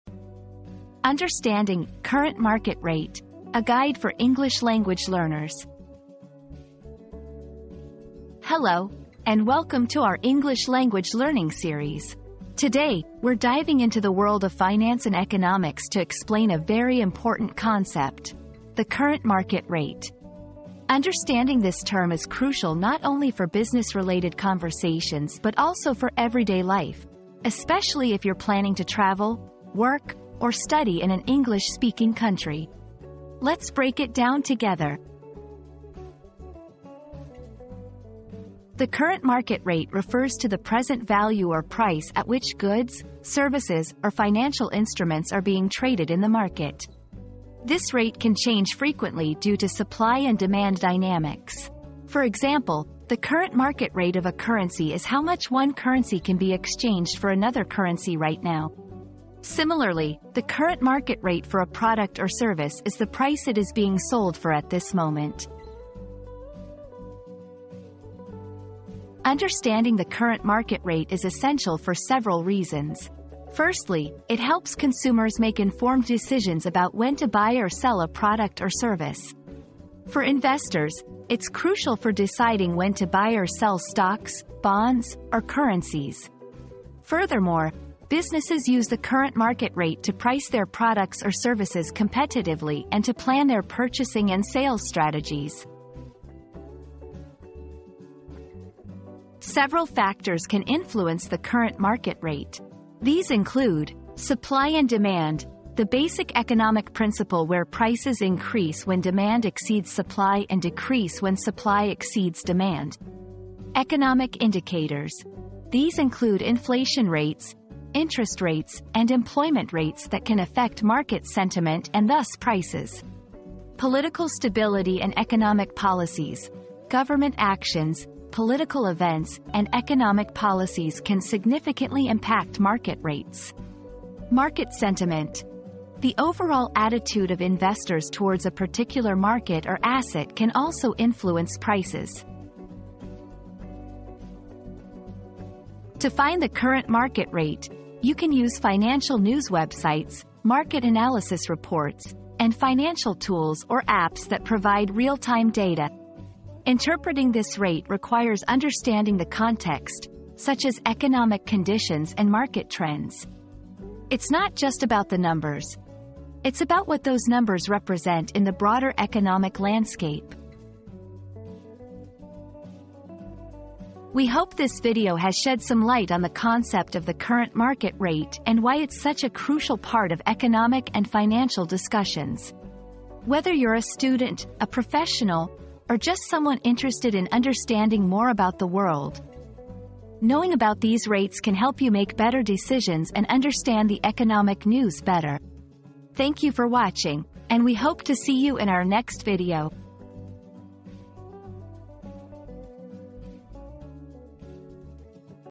Understanding "Current Market Rate": A Guide for English Language Learners
Decoding Market Rates: A Guide for ESL Students • Learn the ins and outs of 'Current Market Rate' in this helpful guide designed for English language learners. Master the terminology and gain a deeper understanding of financial concepts.

00:00 • Introduction - Understanding "Current Market Rate": A Guide for English Language Learners
00:38 • What is the "Current Market Rate"?
01:08 • Why is the Current Market Rate Important?
01:38 • Factors Influencing the Current Market Rate
02:25 • How to Find and Interpret the Current Market Rate